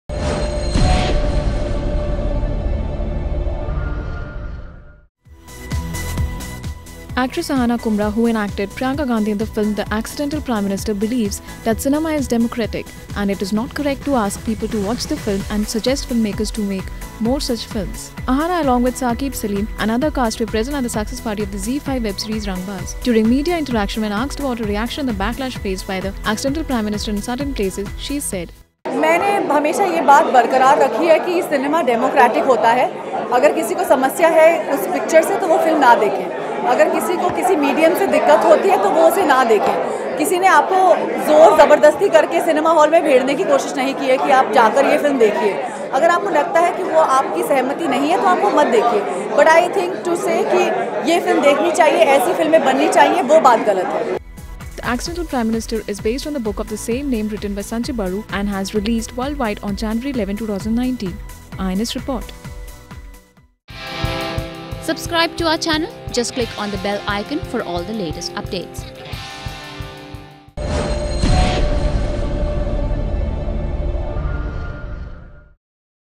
Wrong to advise people to watch 'The Accidental Prime Minister' : Aahana Kumra
Actress Aahana Kumra who enacted Priyanka Gandhi in the film 'The Accidental Prime Minister' believes that cinema is democratic and it is not correct to ask people to watch the film and suggest filmmakers to make more such films.